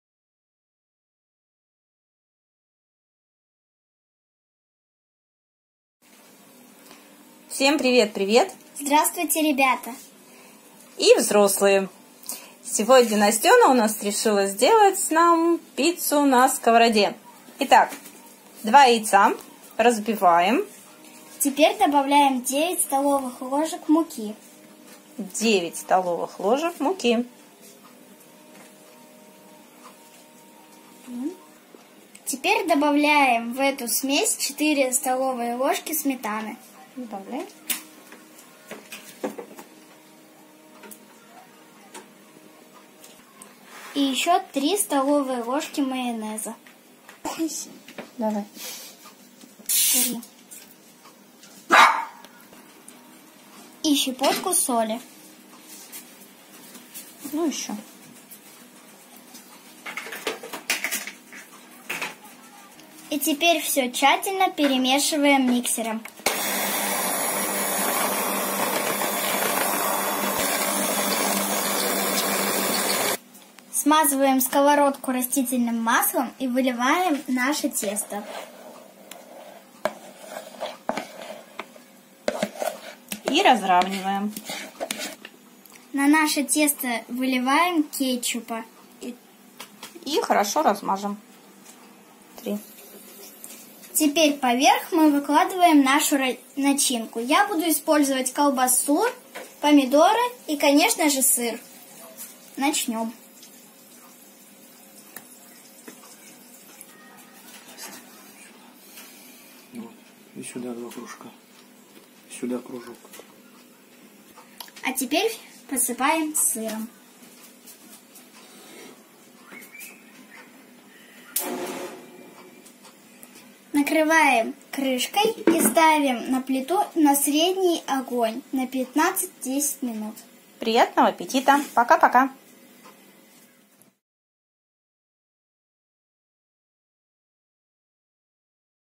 Пицца на сковороде от Настёны😉
2 яйца,4 стол.л сметаны,3 стол л майонеза,соль,9 стол л муки хорошо перемешиваем.
Сковороду смазываем раст.маслом,выливаем тесто,капаем кетчуп и выкладываем начинку.
Закрываем крышкой и готовим минут 10-15.
Приятного аппетита!!!😉🍕🍕🍕